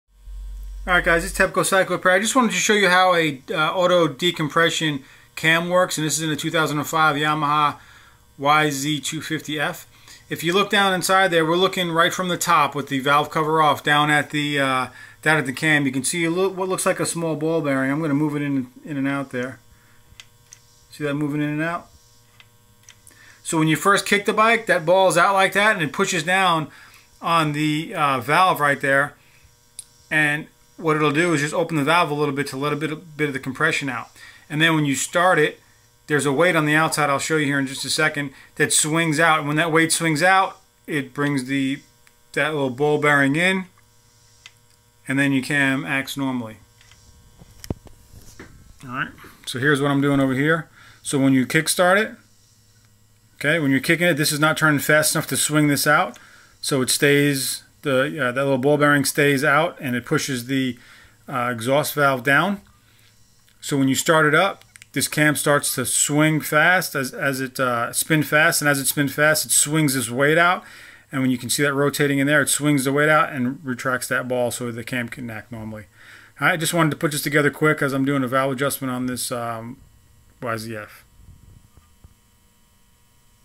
Yamaha Auto Decompression cam, how it works
Just a quick video about how an auto decompression cam works on a 2005 Yamaha YZ250F this cam can be out on older models as well.